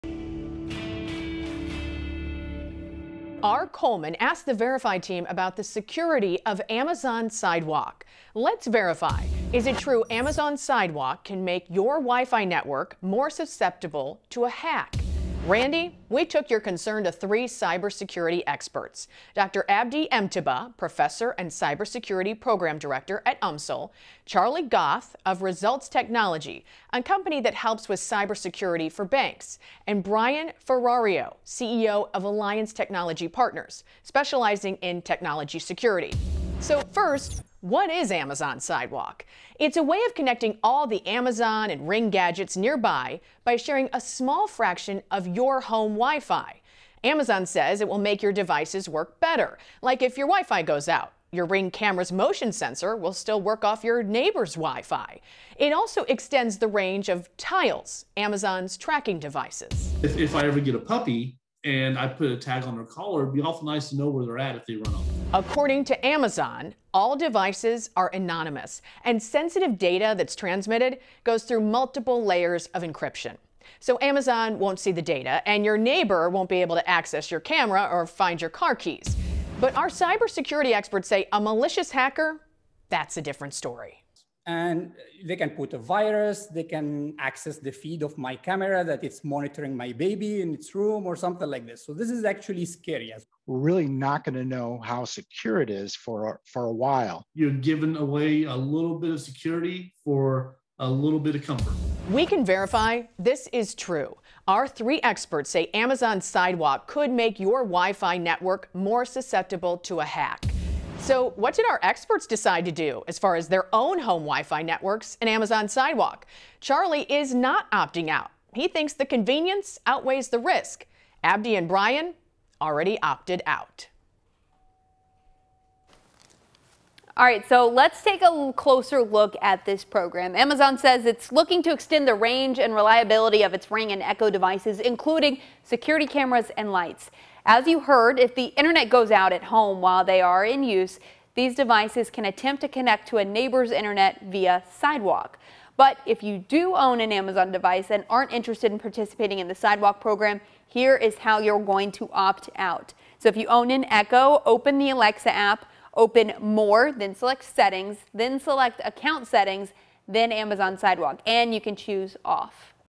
VERIFY: Yes, Amazon Sidewalk can make your Wi-Fi network more susceptible to a hack; How to opt-out
Amazon's new Sidewalk feature has raised concerns about privacy and security.
It also prompted a question to 5 On Your Side's VERIFY team. Viewer R. Kohlman wanted to know about the security of Sidewalk.

Is it true Amazon Sidewalk can make your Wi-Fi network more susceptible to a hack?

True.

All three of our experts agree that participating in Amazon Sidewalk can make your Wi-Fi network more susceptible to a hack.

“They can put a virus, they can access the feed of my camera that is monitoring my baby and through them, or something like this, so this is actually scary as well,” said Mtibaa.

“We’re really not going to know how secure it is for a while,” said Ferrario.

“You’re giving away a little bit of security for a little bit of comfort,” said Goth.

Of our three experts, Mtibaa and Ferrario already opted out. Goth did not opt out of the new feature because he thinks the convenience outweighs the risk.